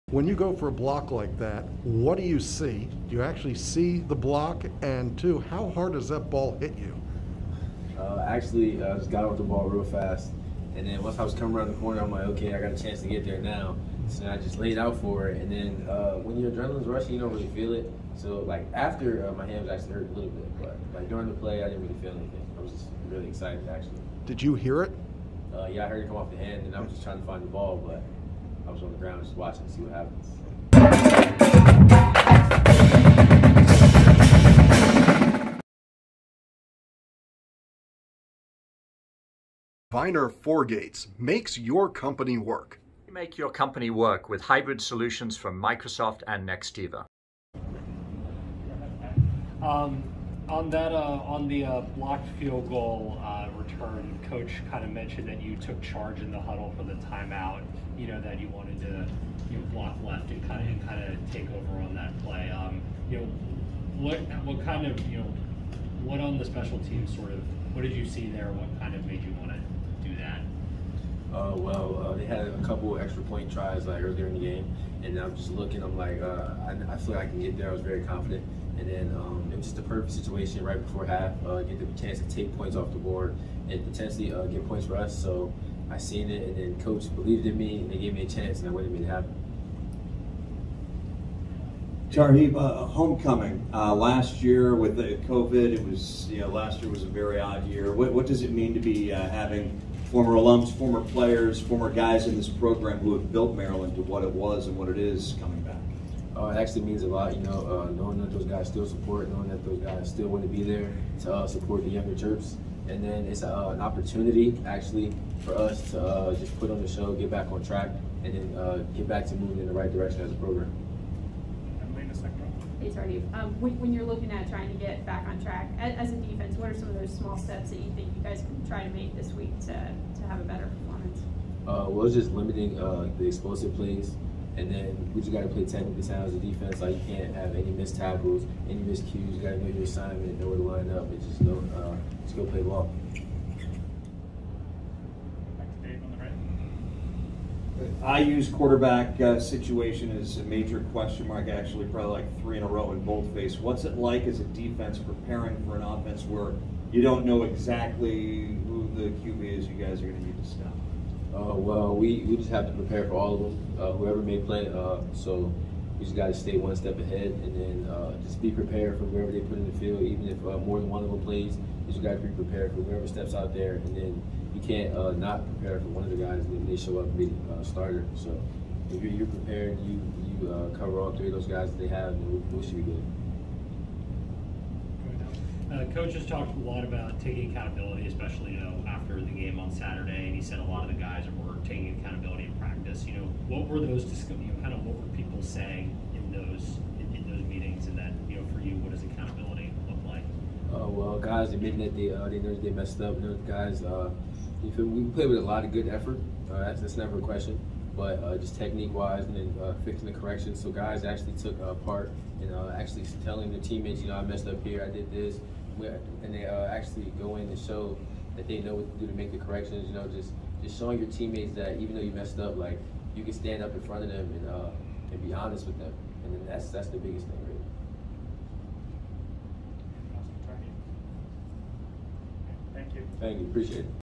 Tarheeb Still Indiana Week - Maryland Football 2021
Star cornerback #12 Tarheeb Still talks about the blocked kick at Minnesota and the upcoming Homecoming game with Indiana.